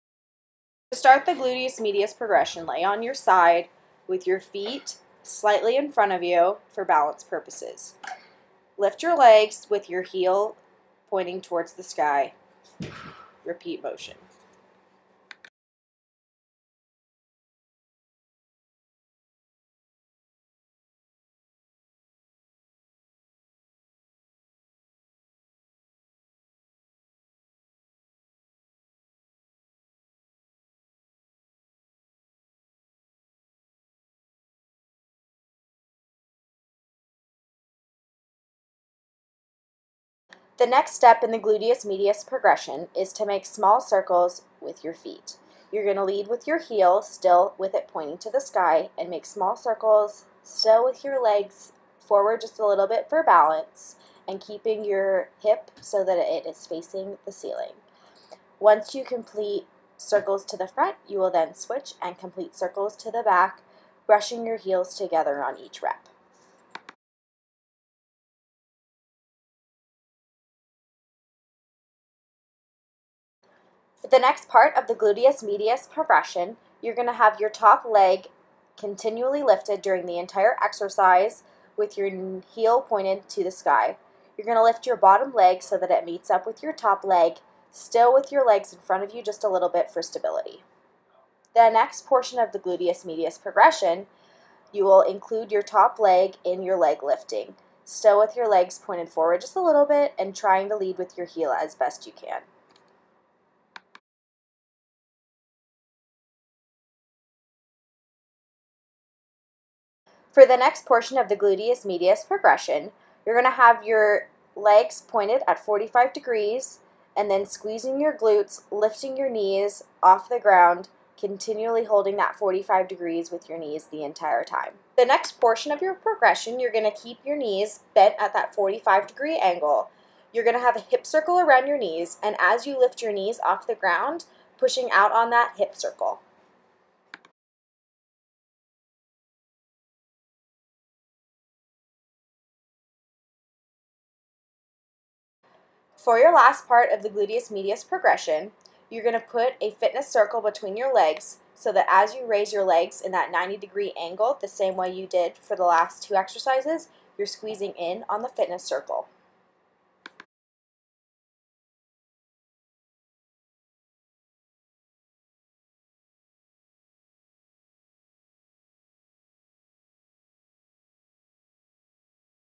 Gluteus Medius Progression
Instructional video on how to complete the gluteus medius warm-up exercises